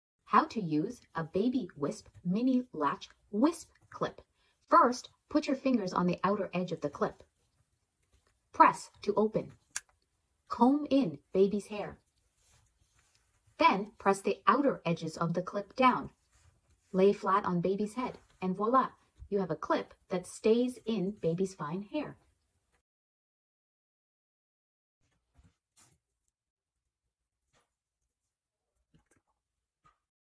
How to Use a Baby Wisp® Mini Wisp Clip for Baby Girl's Fine Hair so the bow stays in!
How do you get a baby hair clip with baby bow to stay in baby girl's fine, wispy hair - use BABY WISP®! Watch this to see the best way to use our mini latch wisp clips for that peach fuzz, 'barely there' hair of infant girls.  Demonstration shows how to open the clip, comb in hair, close the clip - all without disturbing baby girl.  Practise on a friend's arm hair just to see how hairbows can stick - quickly, easily and without leaving marks on baby girl's head!  Baby Wisp® baby and toddler hair accessories have been trusted since 2008 and really work on infant fine hair!